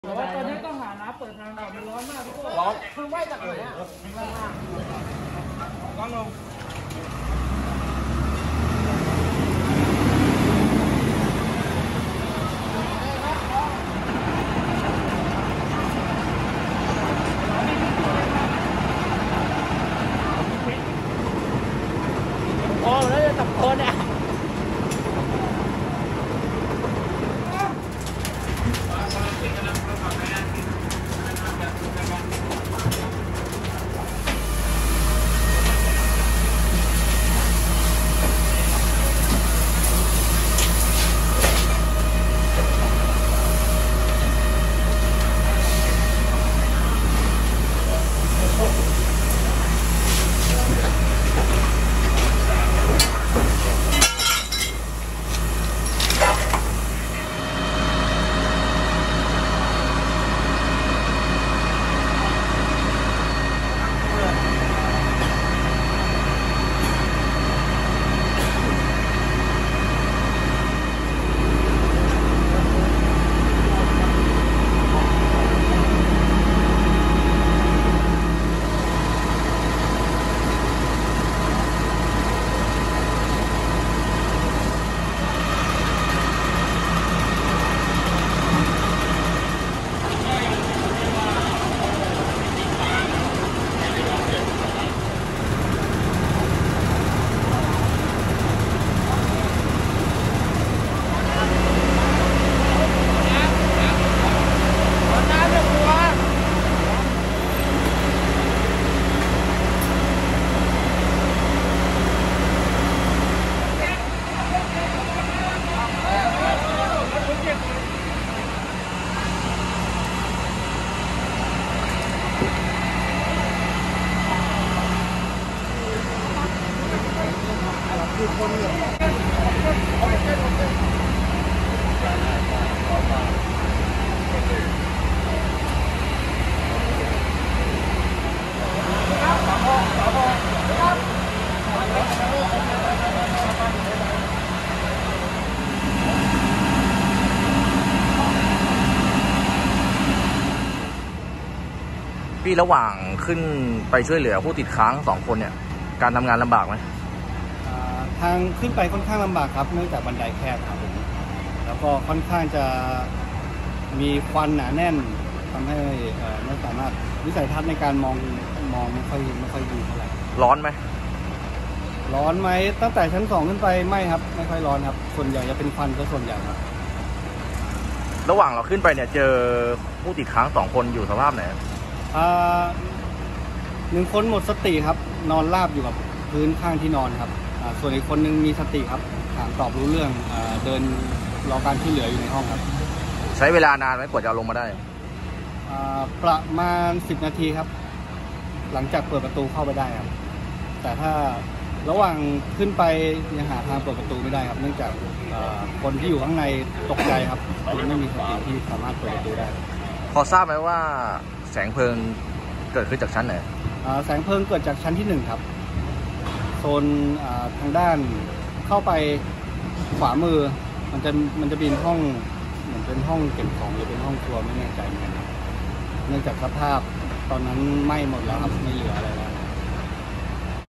เหตุเพลิงไหม้ทาวน์โฮม 3 ชั้นย่านซอยคู้บอล
เมื่อเวลา 04.18 น. วันที่ 3 พ.ค เจ้าหน้าที่ตำรวจ ครบาลคันนายาว ได้รับเหตุเพลิงไหม้บ้านเรือนประชาชน มีพักอาศัยผู้ติดค้างอยู่ในบ้าน ภายในซอยคุ้บอน 6 เขตคันนายาว กรุงเทพฯ
จึงเข้าตรวจสอบที่เกิดเหตุพร้อมเจ้าหน้าที่สถานีดับเพลิงบางเขนและอาสาสมัครมูลนิธิป่อเต็กตึ๊ง
ที่เกิดเหตุอยู่ภายในชอยคู้บอลซอย 6แยก2-2ลักษณะเป็นบ้านป็นทาวน์โฮมสูง 3 ชั้น
พบกลุ่มควันสีดำจำนวนมากพุ่งออกมาจากตัวบ้าน
บริเวณชั้นที่ 1-2 จำนวนมากเจ้าหน้าที่ดับเข้าตรวจสอบภายในบ้านพบแสงเพลิงขึ้นบริเวณชั้นที่ 1 เจ้าหน้าที่รีบเร่งใช้น้ำ 1 หัวฉีด ฉีดสะกัดไม่ให้เพลิงลุกลามอยู่ประมาณ 10-20 นาทีเพลิงสงบเหลือแต่กลุ่มควัน จากนั้นเจ้าหน้าที่ฝ่ายปฏิบัติการในตัวอาคารเข้าตรวจสอบภายในบ้านเบื้องต้นพบผู้พักอาศัยติดค้างอยู่บริเวณชั้น 3 เป็นหญิงสองคน
เจ้าหน้าที่พบว่านอนหมดสติอยู่ 1 คน และอีก 1 คนมีสติเดินอยู่เพื่อรอการช่วยเหลืออยู่จากนั้นเจ้าหน้าที่เร่งให้การช่วยเหลือแต่ในการช่วยเหลือนั้นค่อนข้างลำบากเพราะชั้นที่ 2 ของบ้านมีความร้อนและควันให้เวลาอยู่ประมาณ 30-40 นาทีจึงขึ้นไปชั้นที่ 3 ได้จากนั้นเร่งให้ช่วยเหลือหญิงทั้ง 2 คน ลงมาได้เพียง 10 นาทีและมอบให้กู้ภัยปฐมพยาบาลเบื้องต้นรีบส่งโรงพยาบาลน

จากนั้นเจ้าหน้าที่ยังพบน้องแมวนอนตายอยู่บริเวณชั้นที่ 2 ของบ้าน

ผู้สื่อข่าวสอบถามนายปรีชา  แสนบันบาน  อาสาดับเพลิง ในการขึ้นไปช่วยเหลือผู้ติดค้าง ตนบอกว่า
การขึ้นไป ค่อนข้างลำบากเพราะบันไดแคบมีควัน หนาแน่น ทำให้ วิสัยทัศน์การมองไม่ค่อยเห็นพอขึ้นไปพบผู้ติดค้างตนบอกว่า 1 คนหมดสตินอนราบพื้นข้างที่นอนส่วนอีก 1 คนมีสติถามตอบรู้เรื่องเดินรอการช่วยเหลืออยู่

ส่วนสาเหตุเพลิงไหม้ครั้งนี้เจ้าหน้าที่ตำรวจและผู้เกี่ยวข้องอยู่ระหว่างตรวจ📌📌📌